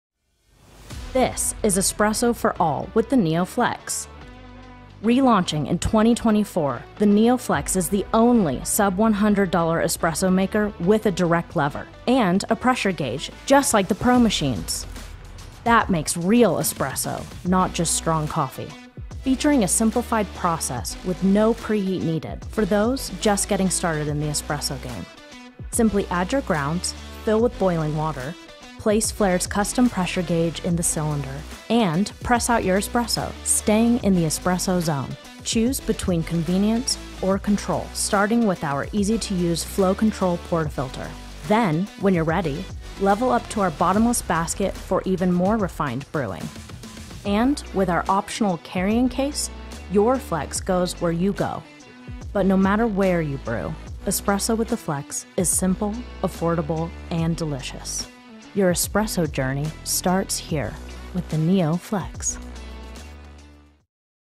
Flair Espresso Maker- NEO Flex 2024新版簡介
歡迎收看Flair Espresso Maker 2024年新版NEO Flex的簡介，你可以由這個影片來初步認識標配壓力錶的新版Flair NEO Flex。

2024年新版NEO Flex有兩個巨大的改善: 1. 標配壓力錶，因此使用者可以立即感知濾壓時的壓力條件，及2. 改為薄壁汽缸，濾壓汽缸熱質量大為降低，因此可以省去預熱的動作及時間。

上面兩大改善，再搭配原本Flex的兩種標配濾杯: 紅色的限流(加壓)濾杯及黑色的無底濾杯，以及Flex本身的便攜性，讓您用最少的花費，就能在任何時空自行製作精品咖啡店等級的Espresso!!

想獲得更多NEO Flex相關資訊，請訪問我們台灣的網站